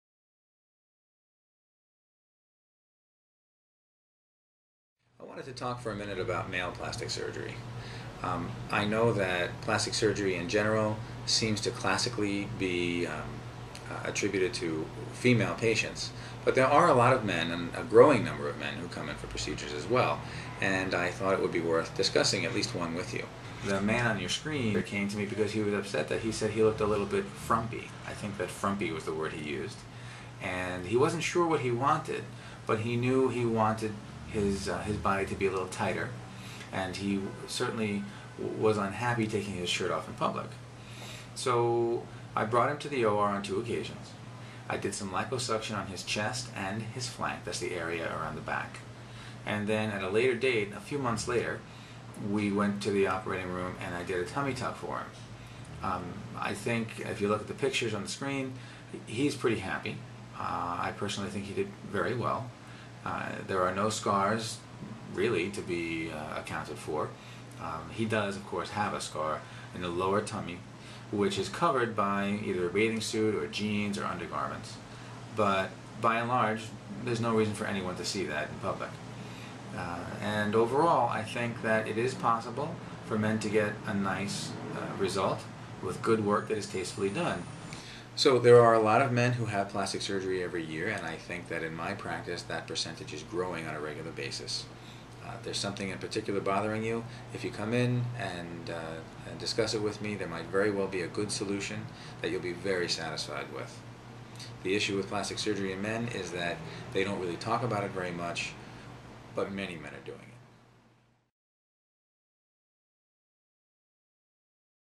Male Plastic Surgery - Case Study | Dr. Sterry
In this video, board-certified plastic surgeon Thomas P. Sterry, MD puts stereotypes at bay when it comes to men and plastic surgery. It is very common for men to seek plastic surgery and continues to gain popularity. He highlights a real patient who received liposuction on his chest and a tummy tuck.

To learn more about the plastic surgery procedures offered at our New York practice, visit our

Thomas P. Sterry, MD
1080 5th Avenue